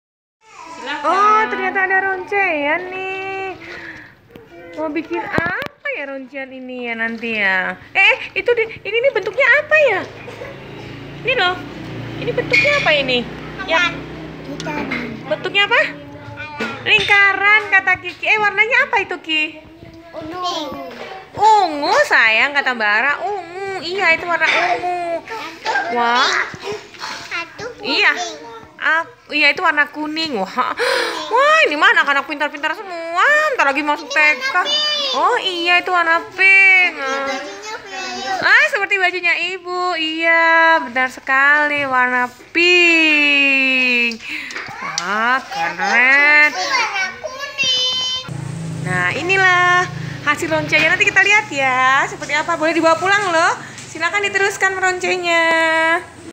Sedotan
Meronce dengan menggunakan sedotan untuk melatih koordinasi mata dan tangan